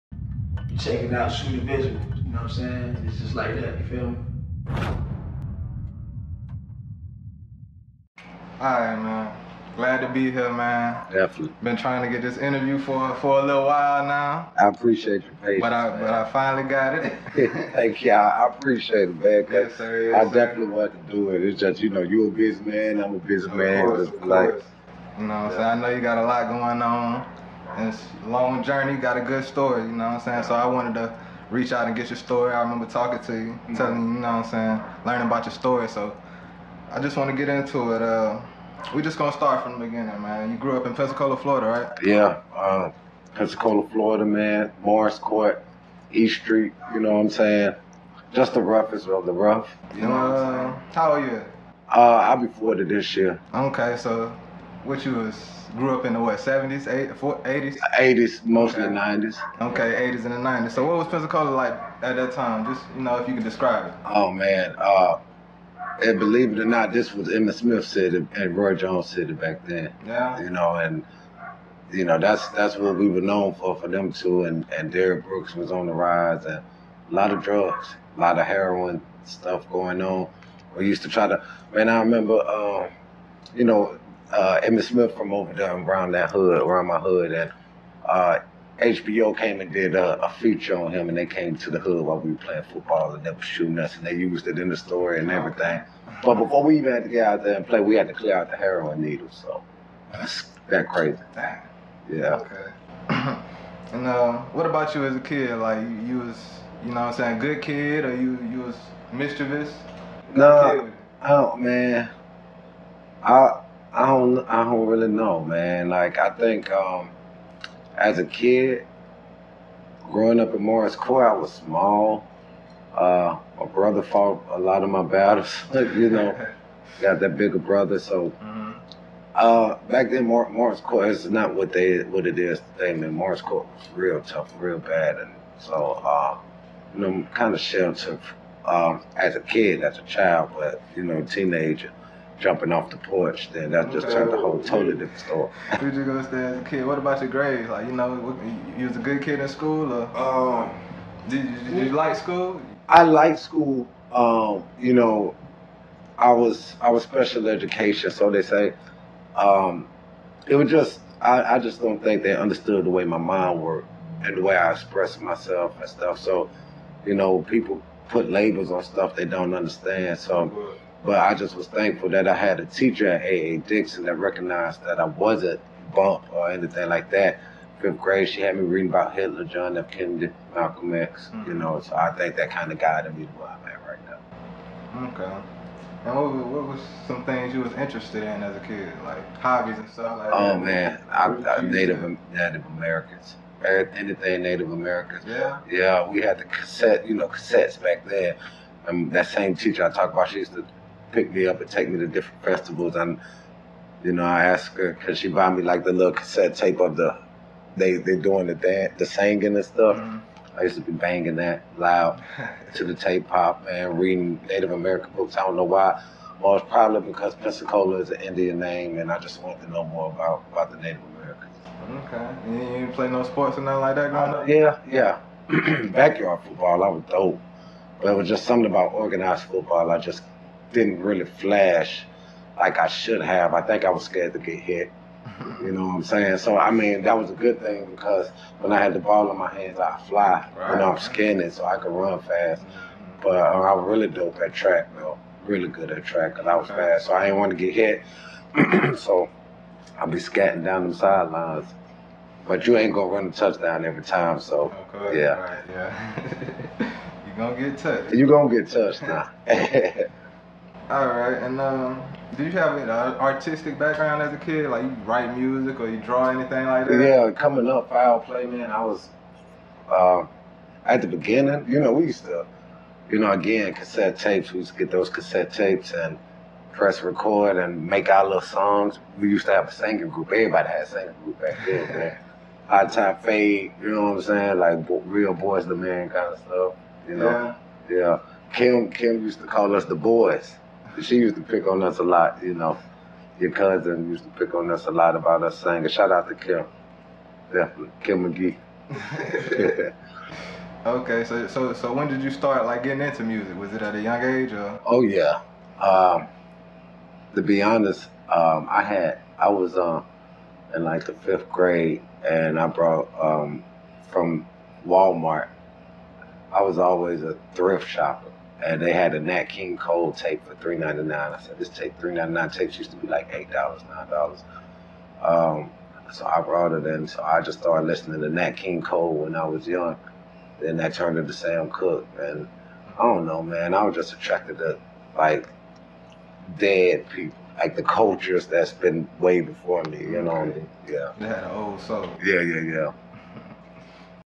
Jermaine Williams: Talks childhood "Before we could play, we had to clear out the heroine needles"
Jermaine J. Williams is a changed man hailing from Pensacola, FL. From being an addict for 13 years to directing an award winning documentary! Nowadays he is Author/Musician/Director/Community Activist all in one and his story is an incredible one. In Part 1 he talks about his life as a kid and some of his interest. But he also mentions the drug abuse that was happening all around the city. Get the full exclusive story here!